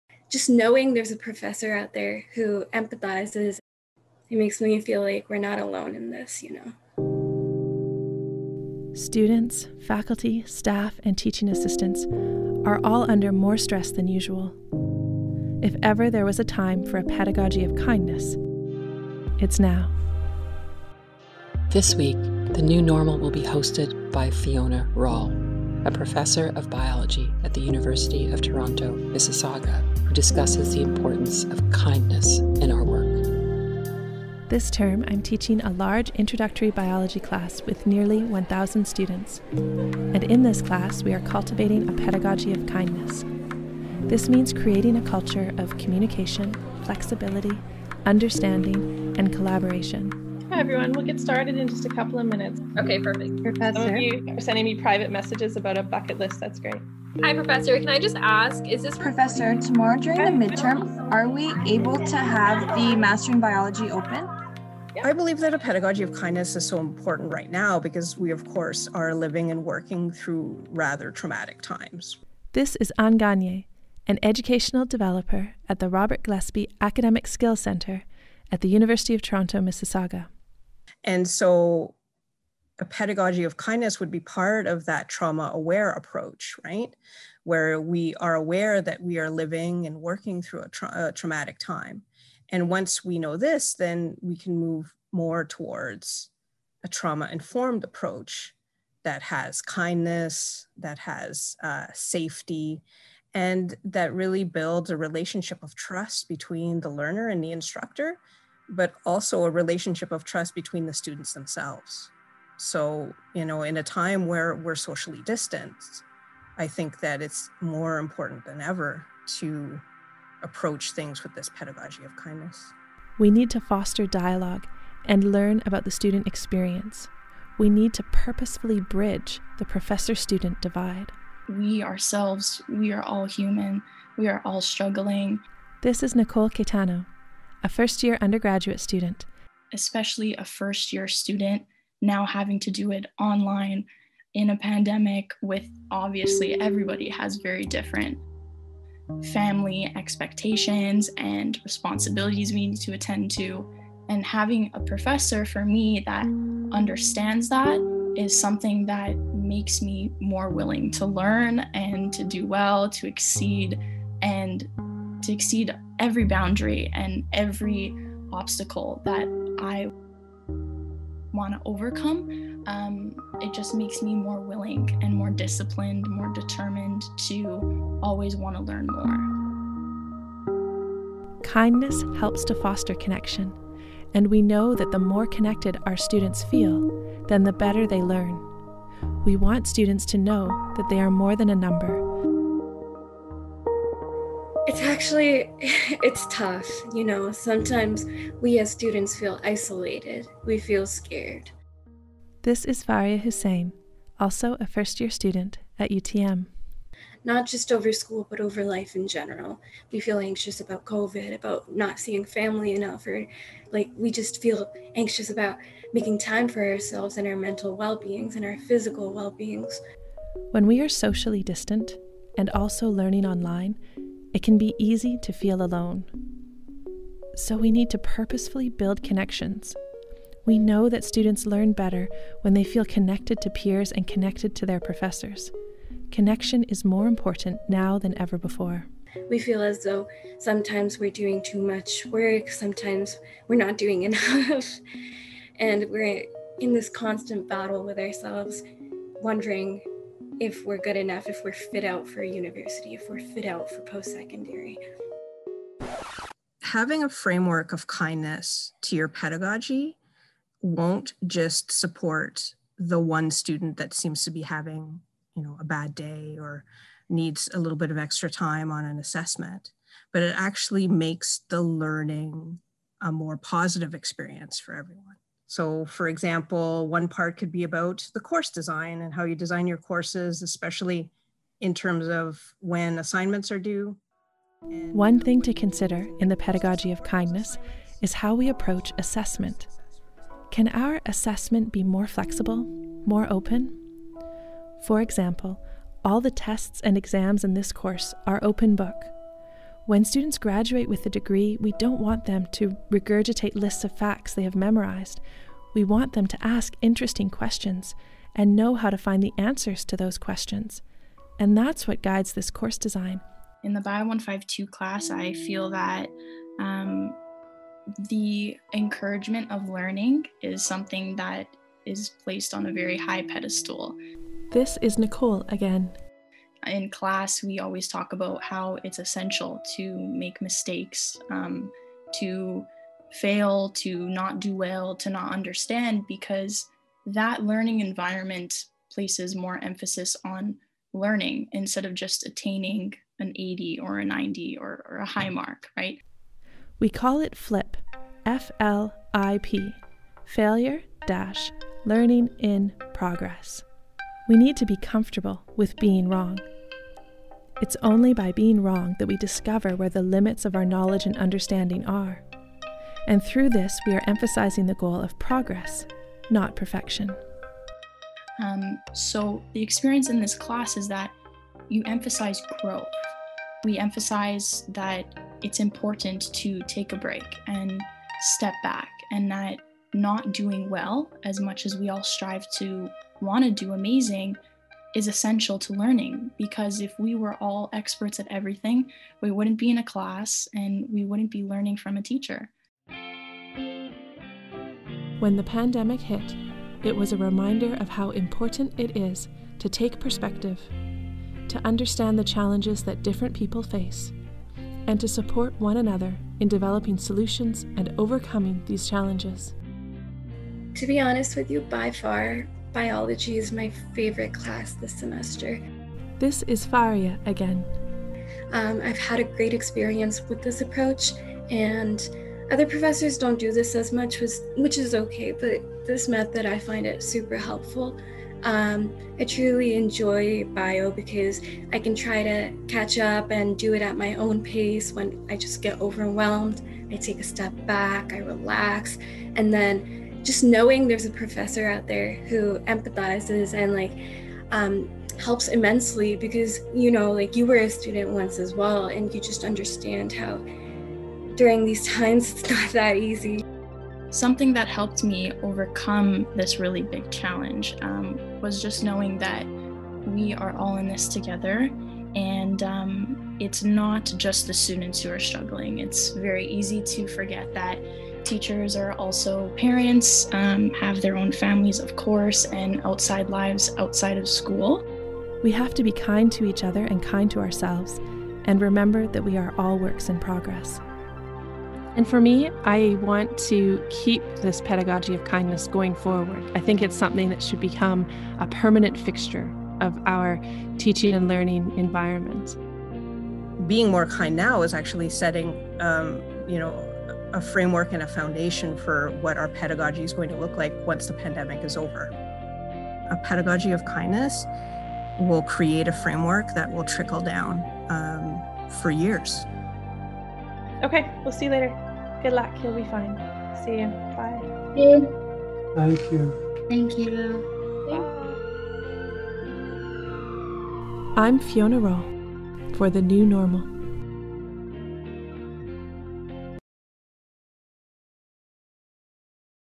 The New Normal with Maydianne Andrade (Ep. 12): The Pedagogy of Kindness
When the pandemic hit, Fiona Rawle – a professor of biology and the associate dean, undergraduate at the University of Toronto Mississauga – was quick to analyze the challenges of remote teaching and learning and determine that a new approach was needed.

“Students, faculty, staff, and teaching assistants are all under more stress than usual,” Rawle says. “If ever there was a time for a pedagogy of kindness, it's now.”

Rawle designed and co-led a series of workshops to help faculty members better connect with students and provide them with the necessary support. The goal is to create “a culture of communication, flexibility, understanding and collaboration,” she says.

“We need to foster dialogue and learn about the student experience. We need to purposefully bridge the professor-student divide.”

In episode 12 of The New Normal podcast with Maydianne Andrade, guest host Rawle speaks with undergraduate students Nicole Caetano and Faarea Hussain about the challenges they’re facing during the pandemic. She also speaks with Ann Gagné, an educational developer at the Robert Gillespie Academic Skill Centre at U of T Mississauga.

“I believe that a pedagogy of kindness is so important right now because we, of course, are living and working through rather traumatic times,” says Gagné. “Once we know this, then we can move more towards a trauma-informed approach that has kindness, that has safety and that really builds a relationship of trust between the learner and the instructor, but also a relationship of trust between the students themselves.

In the end, Rawle says, kindness helps to foster connection.

“We know that the more connected our students feel, then the better they learn. We want students to know that they are more than a number.”

The New Normal is created in collaboration with a University of Toronto Communications team led by Lisa Lightbourn. You can listen to the podcast on Spotify or listen on SoundCloud. You can also find it on Apple or listen on Google.